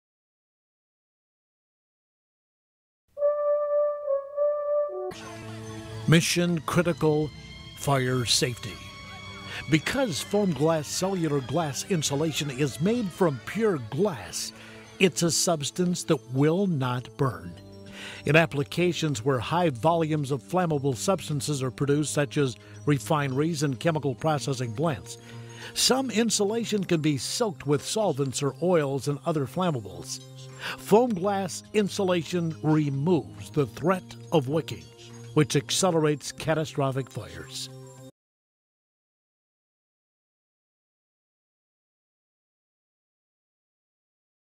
WVSN Radio Discusses Catastrophic Industrial Fires Caused by Solvent Soaked Insulation
Catastrophic Industrial Fires